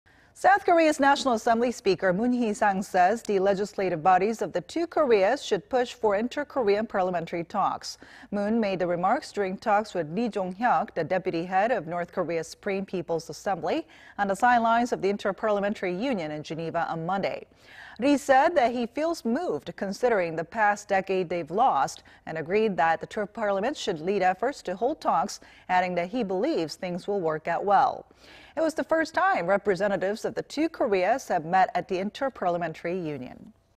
Parliamentary representatives from two Koreas hold talks on sidelines of Inter-Parliamentary Union
文의장, IPU총회서 北리종혁 면담

South Korea's National Assembly Speaker Moon Hee-sang says. the legislative bodies of the two Koreas should push for inter-Korean parliamentary talks.
Moon made the remarks during talks with Ri Jong-hyok  ,... the deputy head of North Korea's Supreme People's Assembly,... on the sidelines of the Inter-Parliamentary Union in Geneva on Monday.
Ri said that he feels moved considering the past decade they've lost,... and agreed that the two parliaments should lead efforts to hold talks,... adding that he believes things will work out well.
It was the first time representatives of the two Koreas have met at the Inter-Parliamentary Union.